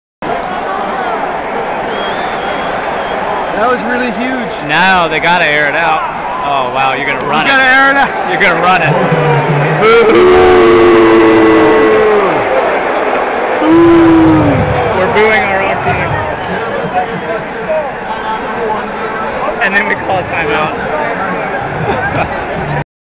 Texas / UCLA final minutes
Not sure why they broke down - Texas has always been a short yardage team, but when you're down 3 possessions with under 6m and starting a drive from around the 10?  Yea, no one was happy about it